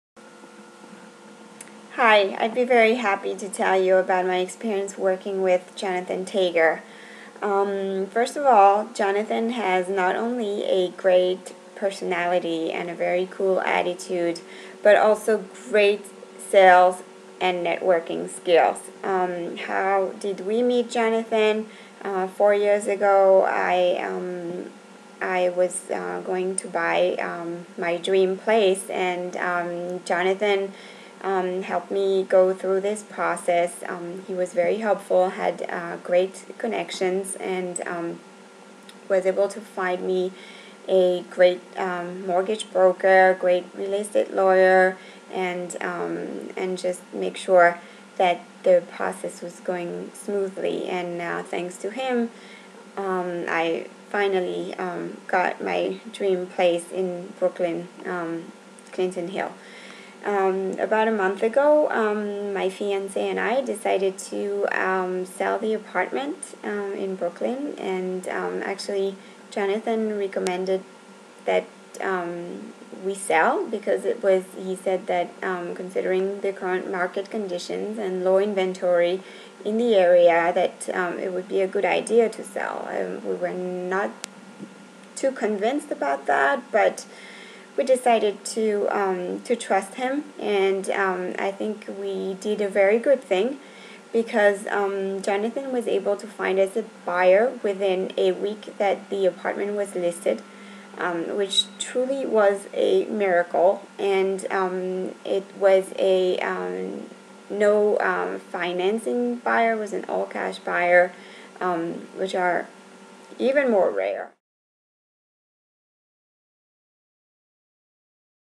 Sophie's Testimony
Sophie talks about her experience with Jonathan Tager